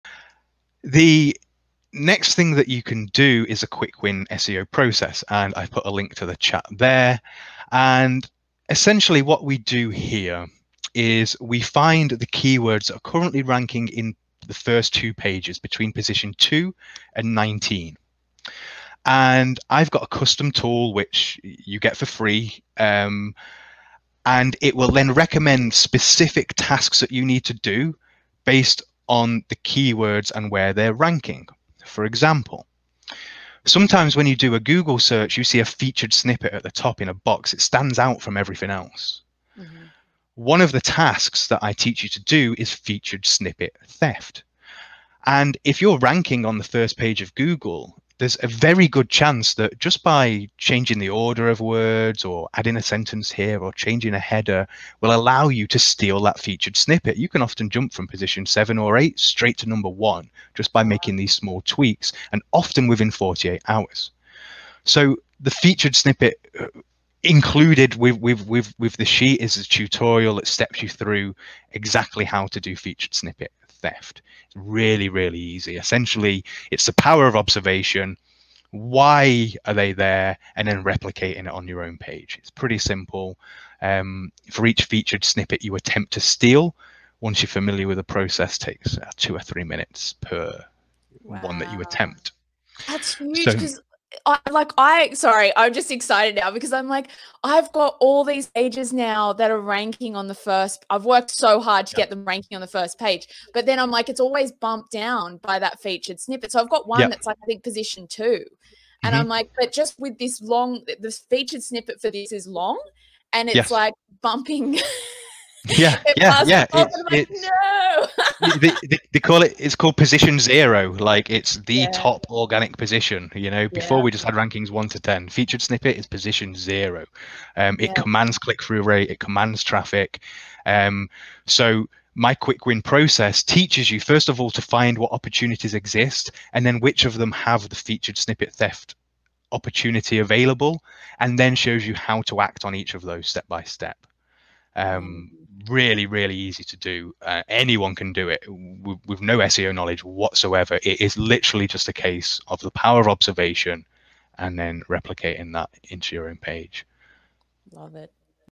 How To Get the #1 Position In Google By Stealing Featured Snippets
📺 In this video, you'll learn how my Quick Win SEO process can help you steal featured snippets in Google.

Why should you watch it?

Well, if you're stuck at the bottom of the 1st page (or even the 2nd page) in Google, you could easily get to the top of the search result within a few changes to your website...

Sounds good?